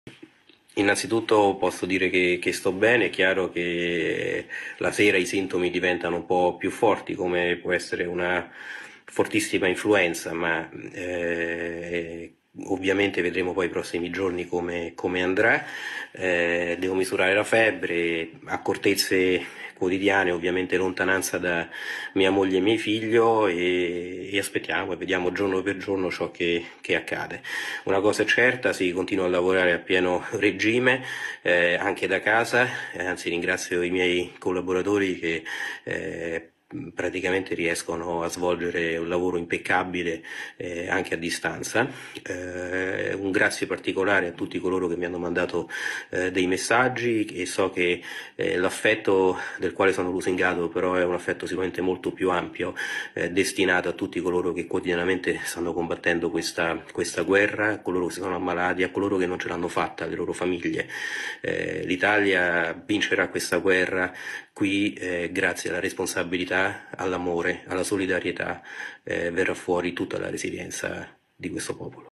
Il viceministro della Salute Sileri: "Sto meglio, l’Italia vincerà questa guerra"
(Agenzia Vista) - Roma, 14 Marzo 2020 - "Sto meglio, l’Italia vincerà questa guerra",queste le parole del viceministro della Salute, Pierpaolo Sileri, in una diretta dal suo profilo Facebook. Facebook Pierpaolo Sileri